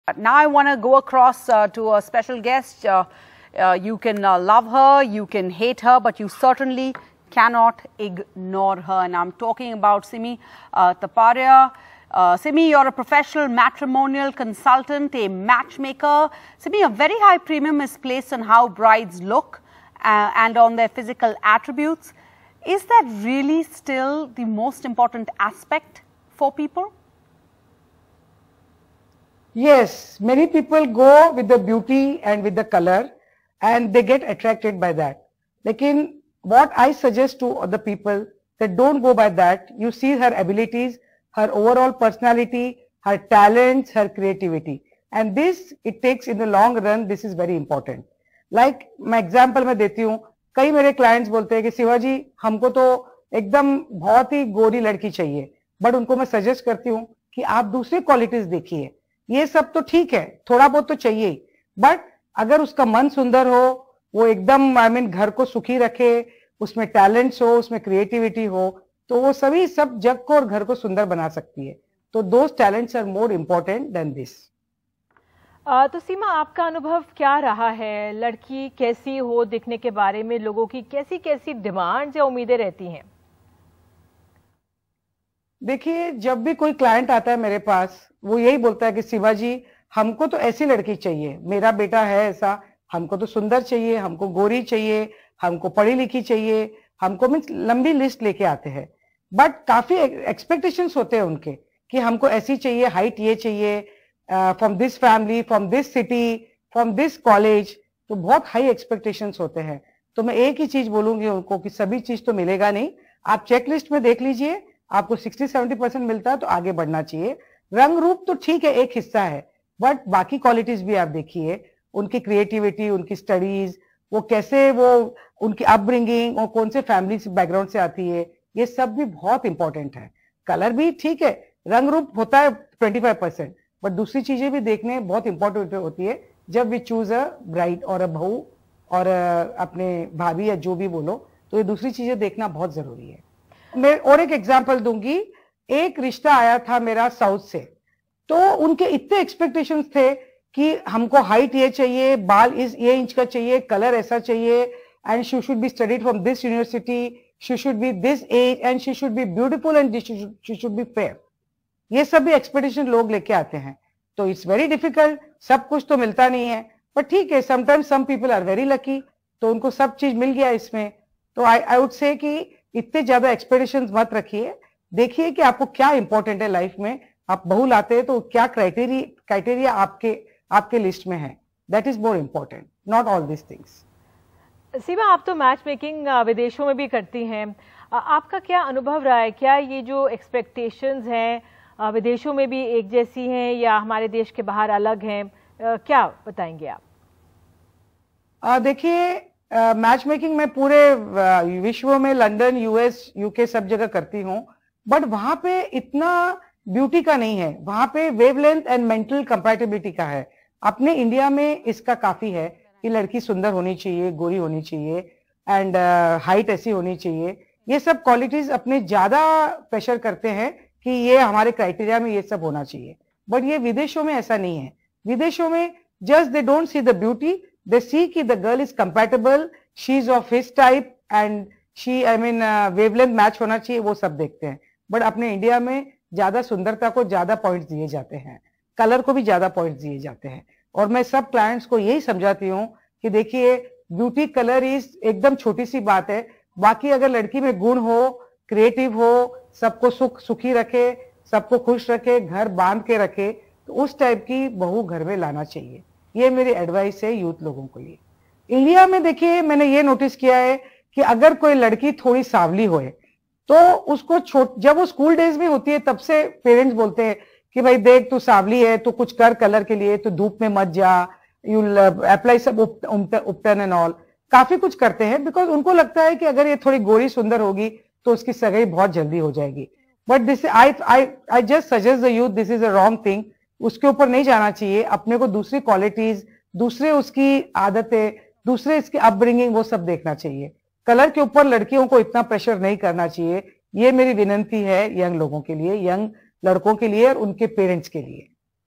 Many Clients Come With A Request Of A 'Fair Complexion': Sima Taparia
[Brand Amp] Sima Taparia a matrimonial consultant narrates different instances where she has encountered stereotypical views of beauty in her profession. She said, "Many people go with the beauty and colour, but I guess look beyond that, the woman's overall personality, talents and intelligence. Many clients come with a request of a 'fair complexion' girl, but I suggest that her nature and personality will beautify the house and not her colour."